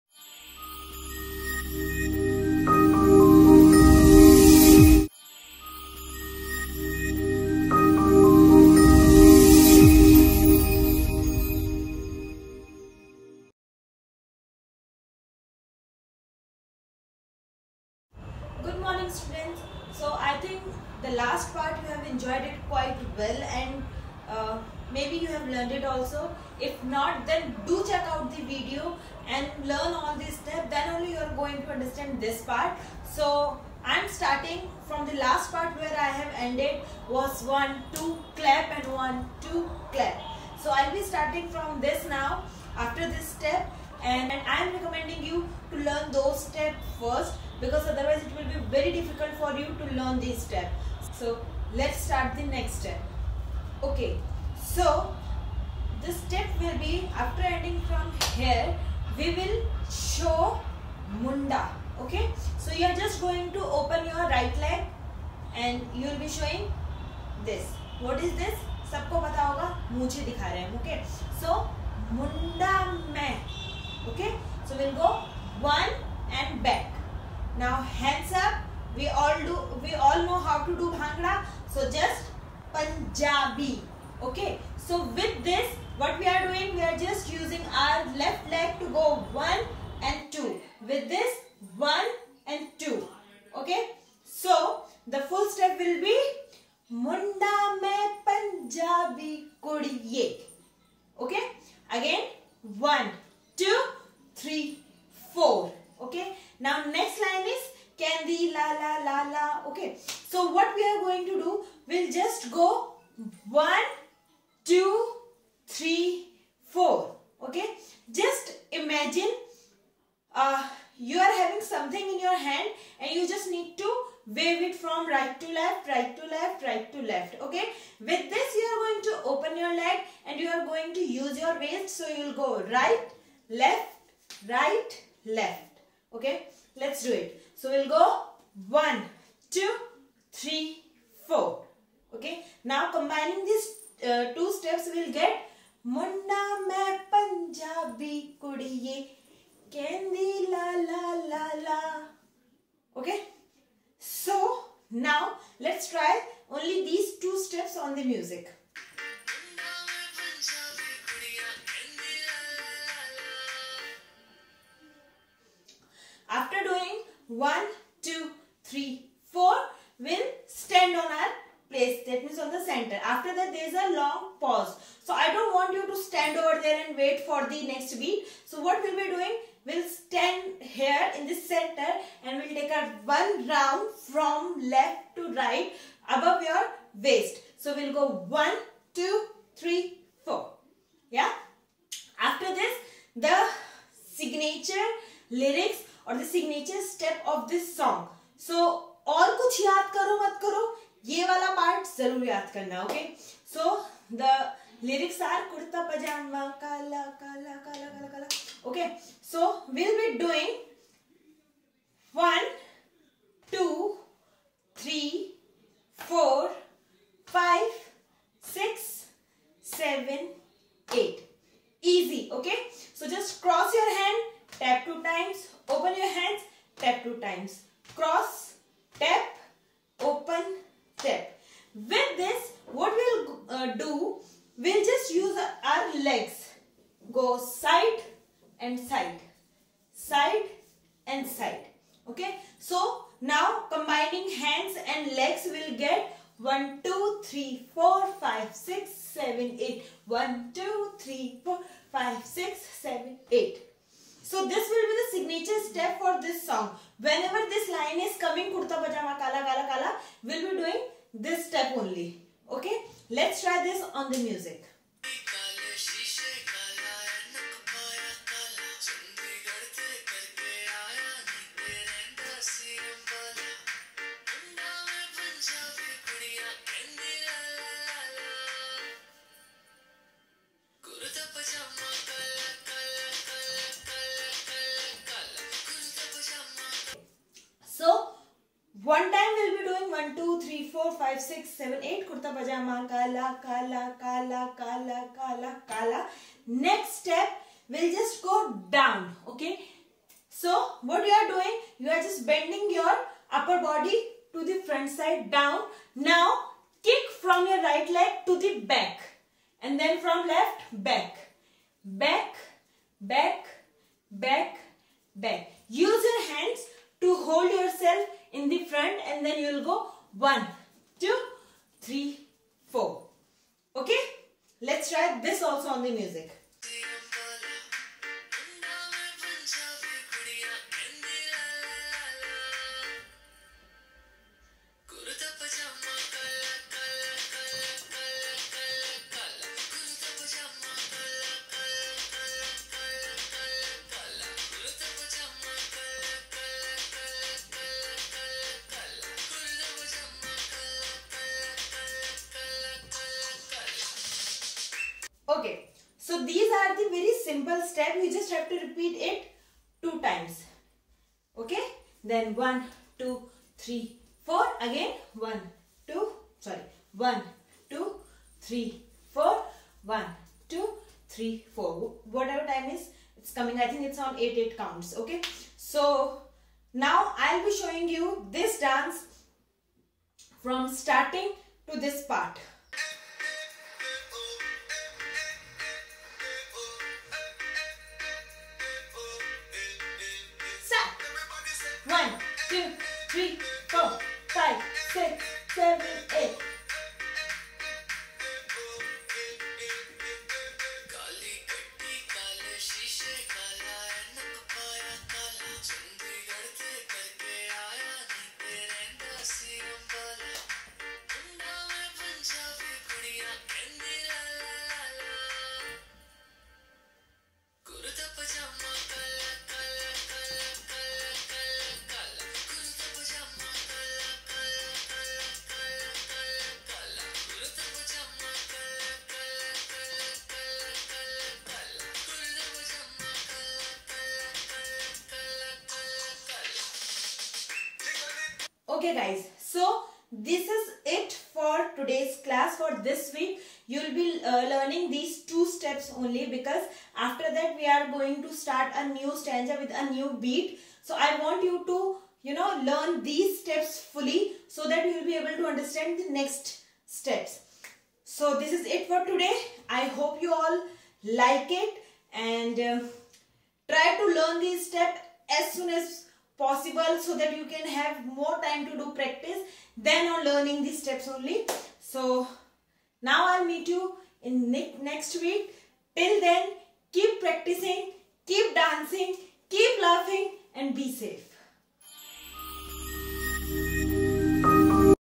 Dance Class Part II// How to learn dance//Dance tutorial/Online Dance Class
Dance tutorial for students/Simple Hip Hop steps for beginners